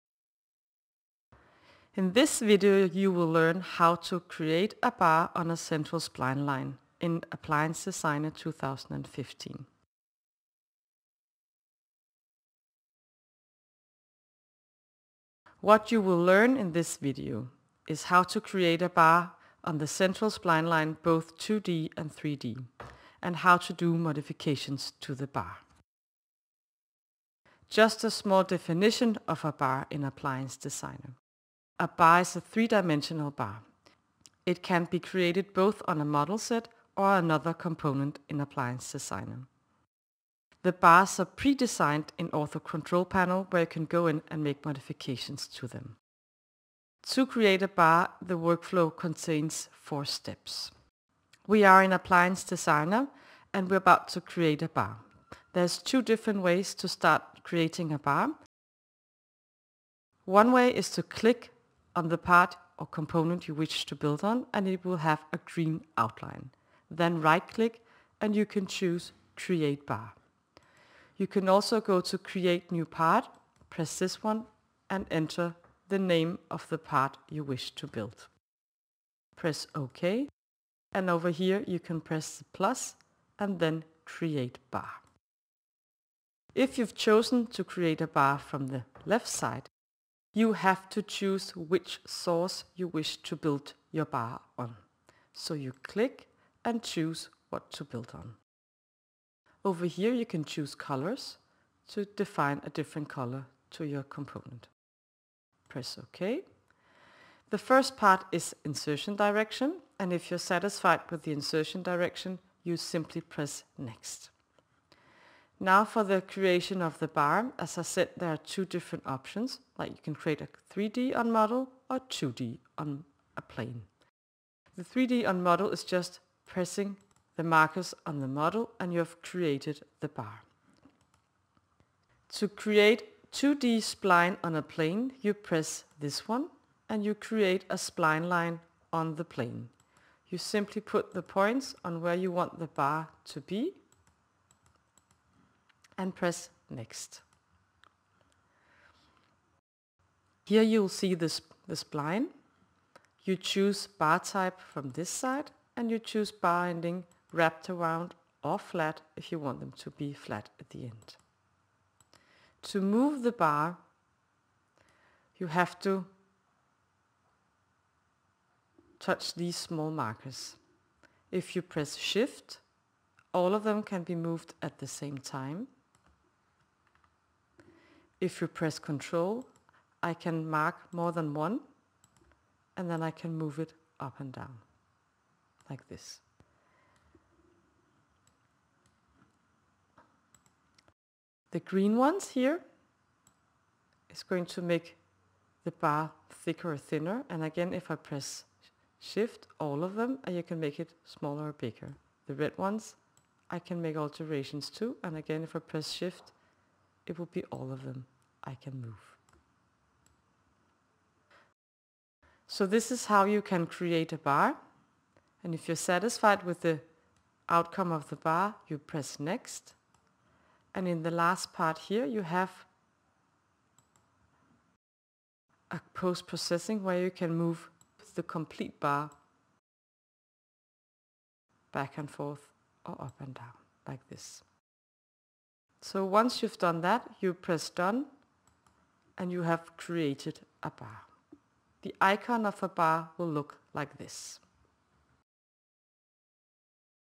3Shape Appliance Designer - How to Create a Bar on Central Spline Line
By watching this video, you will learn how to create a bar on the Central spline both on 2D and 3D. You will also be informed how to do modifications to the bar.